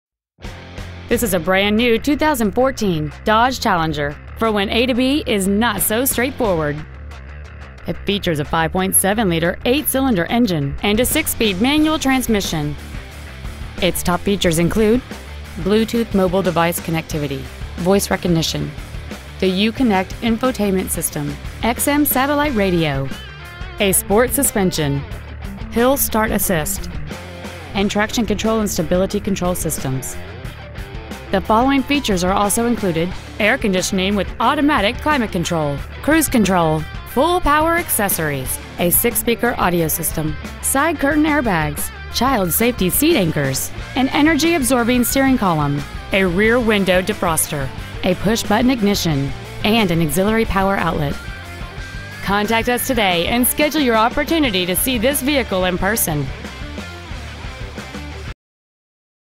2014 Dodge Challenger Rocky Mount NC
Year : 2014
 Make : Dodge
 Model : Challenger
 Engine : 5.7L 8 Cyl. Fuel Injected
 Trans . : 6-Speed Manual
 Exterior : Redline 3 Coat Pearl
 Miles : 687

 1800 N. Wesleyan Blvd

 2014 Dodge Challenger Rocky Mount NC